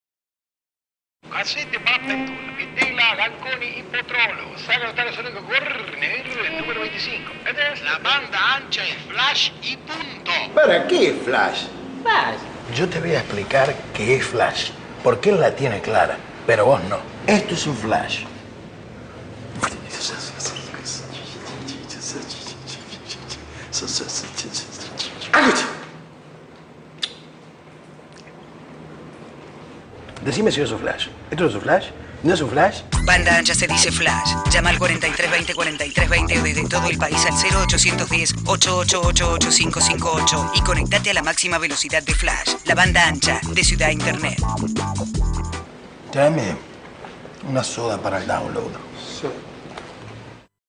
Flash Banda Ancha ALFREDO CASERO 2
Parte de la campaña realizada para proveedor de banda ancha con Alfredo Casero